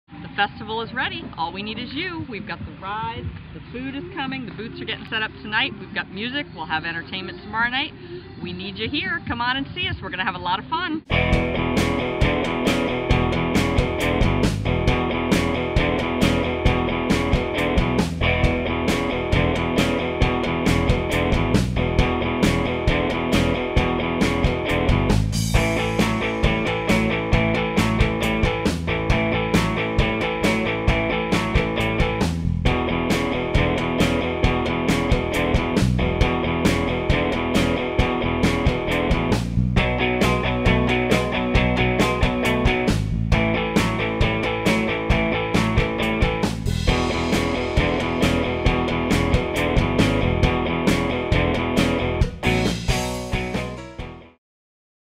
It's St. Raphael Festival time!!!
At last, it's time for the St. Raphael Festival, a three-day, fun-filled extravaganza, with carnival rides for children of all ages, delicious specialty food booths, two themed dinner nights (Oktoberfest and Family Spaghetti Night), live music and Bingo.